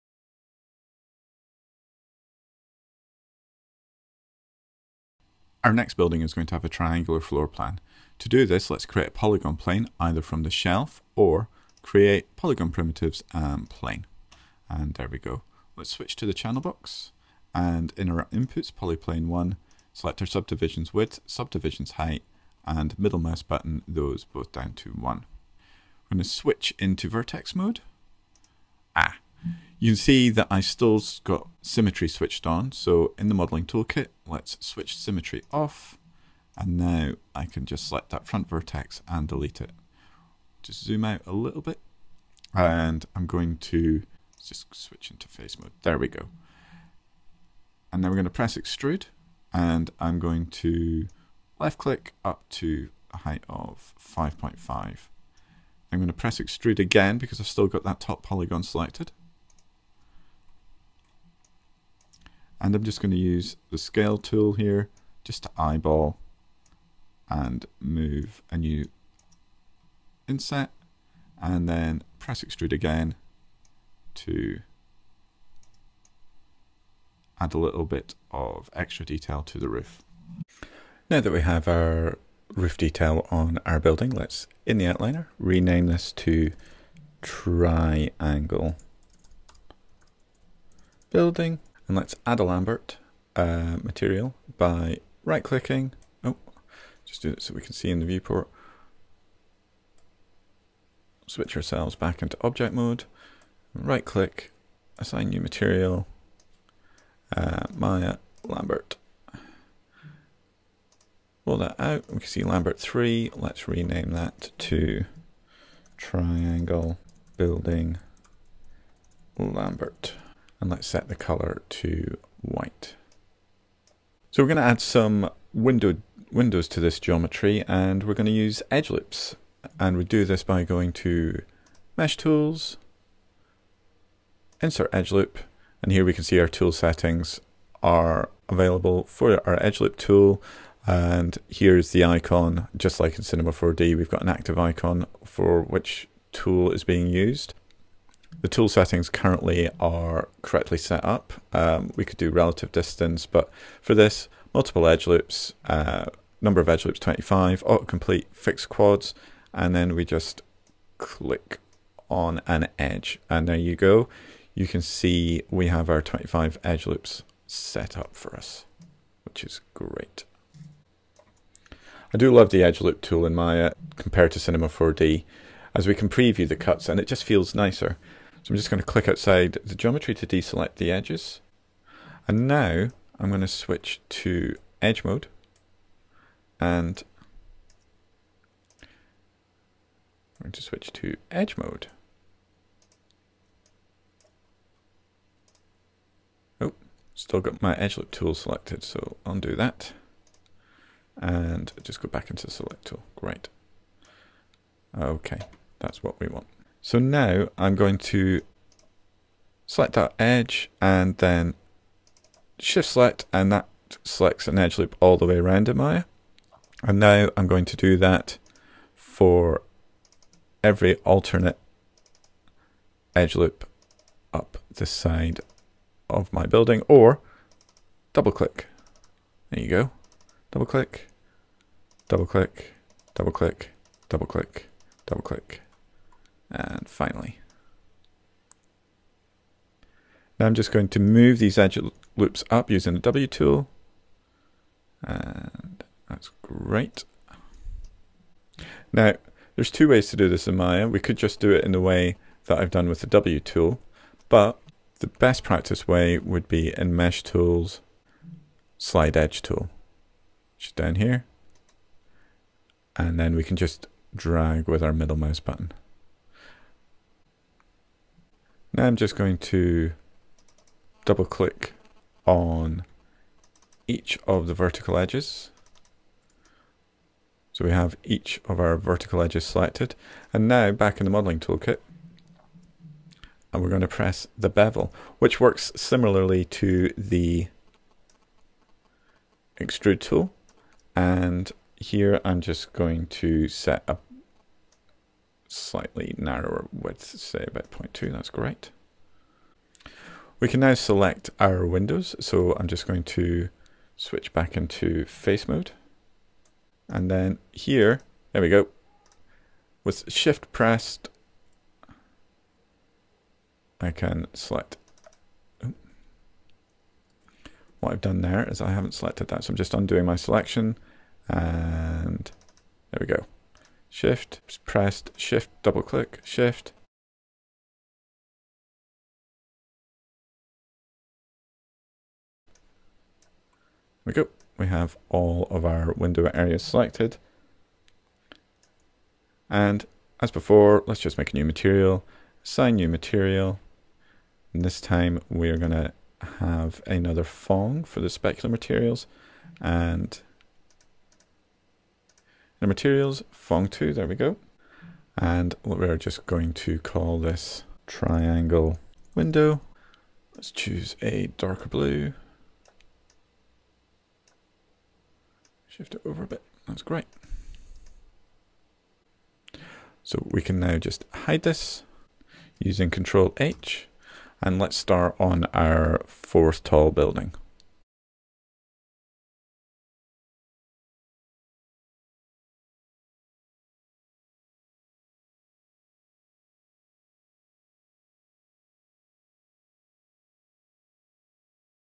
Cinema 4D to Maya 2017: Growing a City - Part 4 - Modelling the third building
In this series, Cinema 4D expert Mike Griggs shows you how to use Maya's MASH toolset to create a growing city effect.

This movie focuses on building the third of a few different polygon buildings.